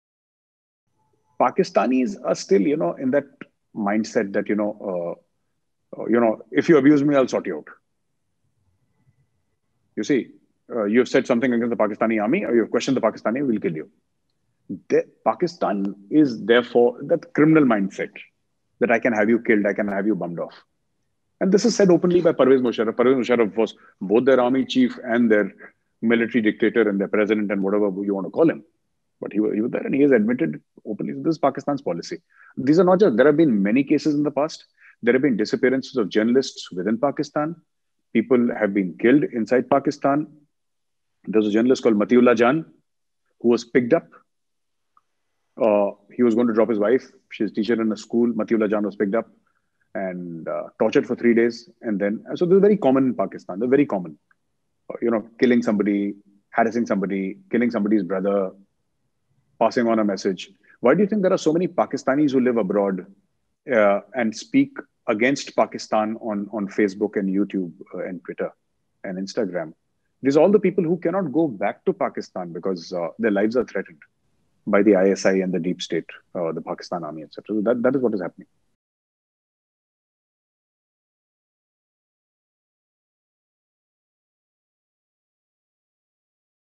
Pervez Musharraf has openly let out that Pakistan is a criminal police state | Major Gaurav Arya ji
Pervez Musharraf has openly let out that Pakistan is essentially a criminal police state | Major Gaurav Arya ji